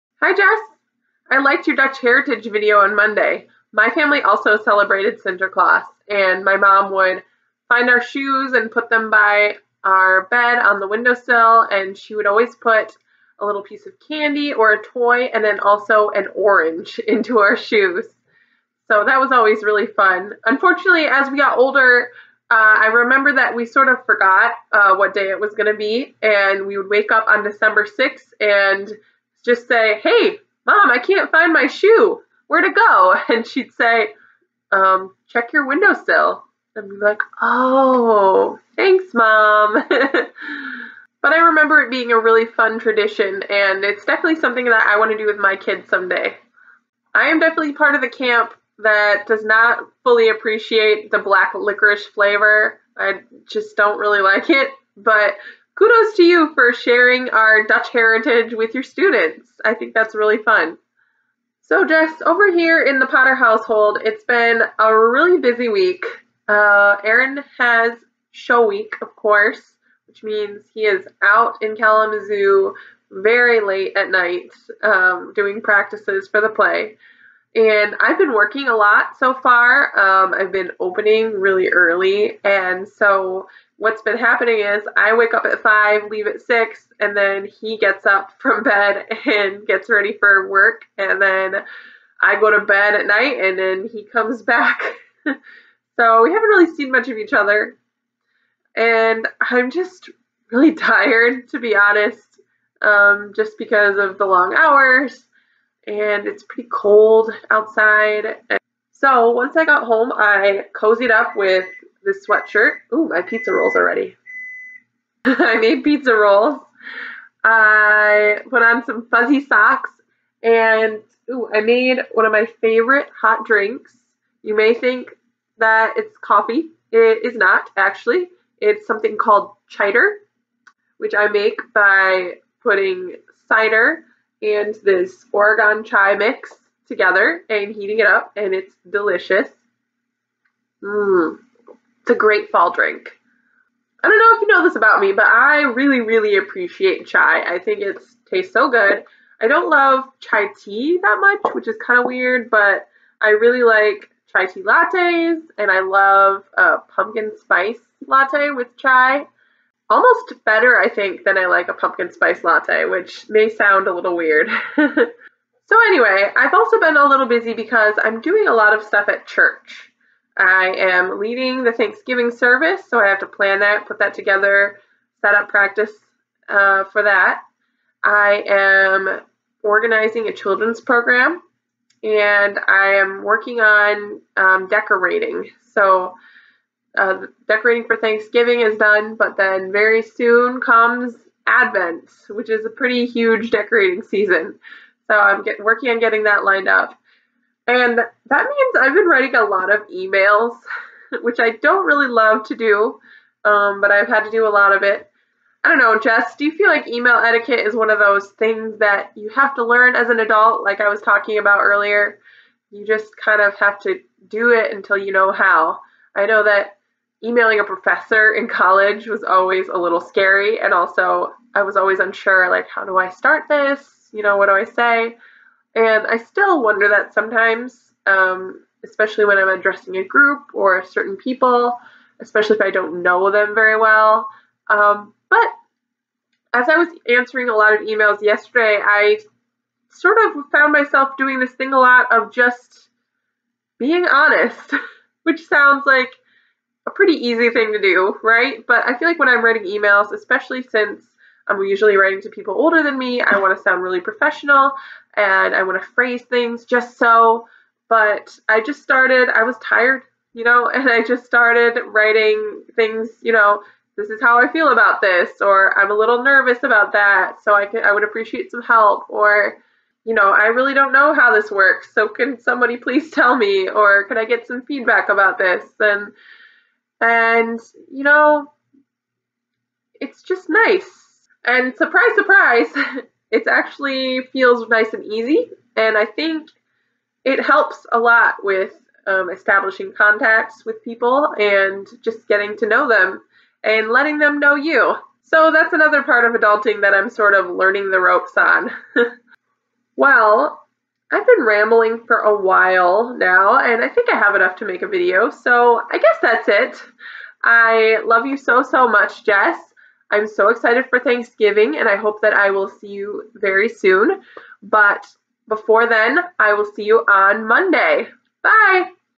Sinterklaas, Coziness, and Emails
Literally just a rambling talk about what's going on in my life this week!